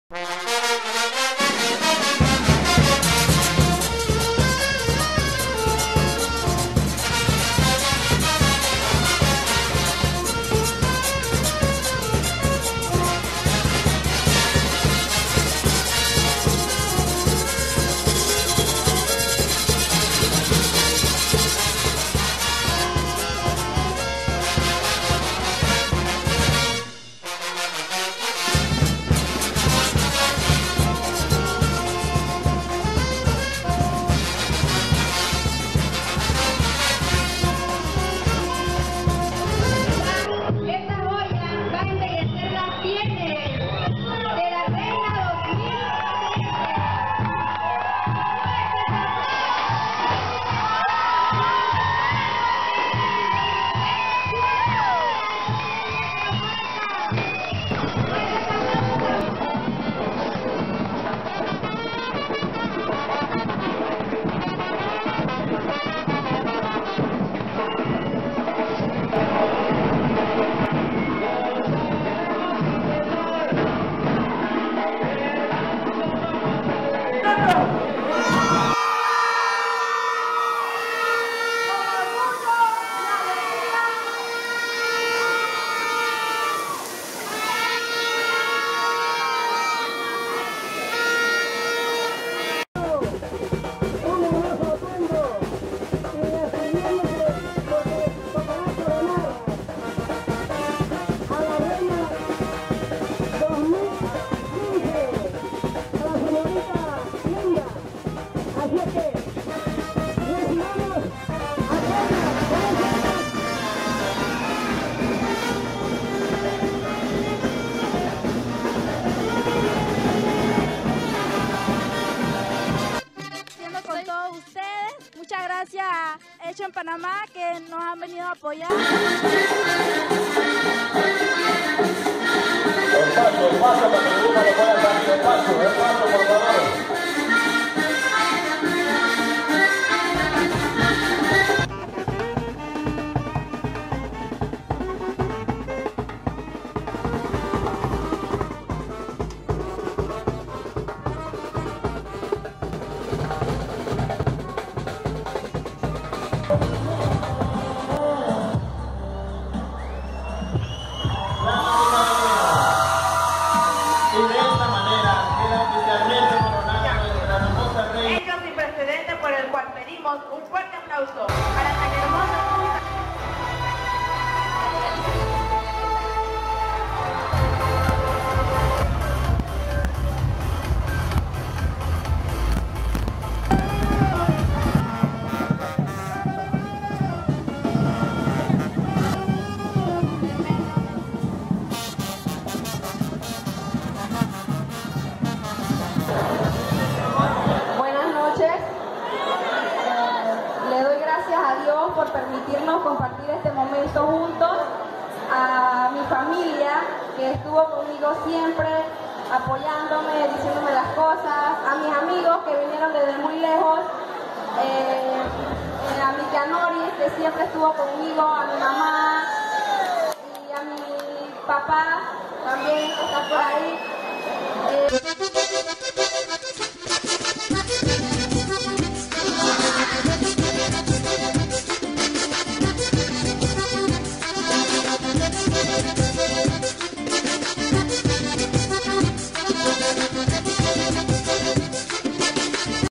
Coclesito
El Grupo CoclesitoColon, les trae un recuento de las REINAS 8 DE AGOSTO, anteriores, encontradas en nuestra gran base de datos de imágenes, y por aproximarse las actividades de aniversario del pueblo, en sus 49 años de fundación.
PD: La Reina del periodo 2011, NO le encontramos foto a pesar de hacer los esfuerzo en conseguirla, pero esta mencionada en el grupo de reinas.